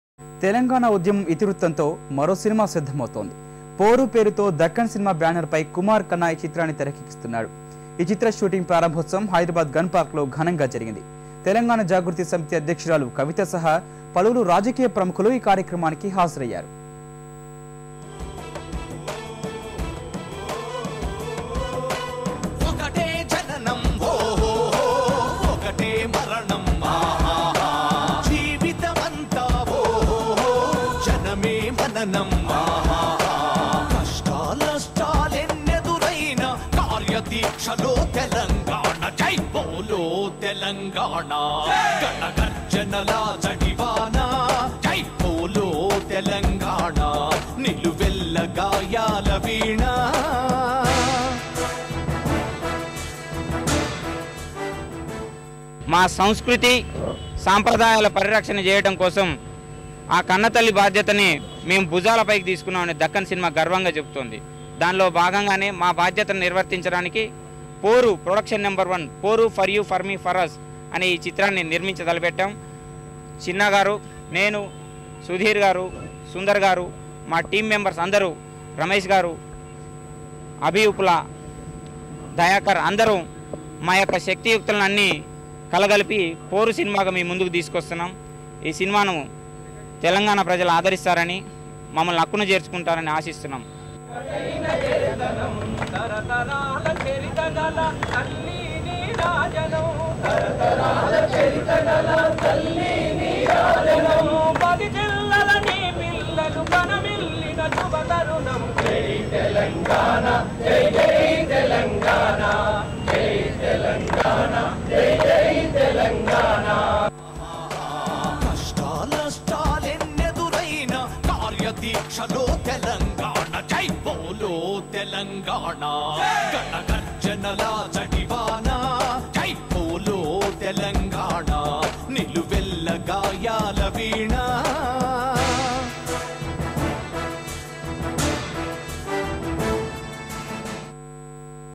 Another movie on Telangana movement titled as Poru
Another movie on Telangana movement titled as 'Poru for u - for me - for us', the low budget feature film is being produced to depict how youngsters who are strange to the movement throw their lot without even knowing about the struggle. The star cast includes amateur artistes led by Krishank, the spokesperson of a student organisation in Osmania University.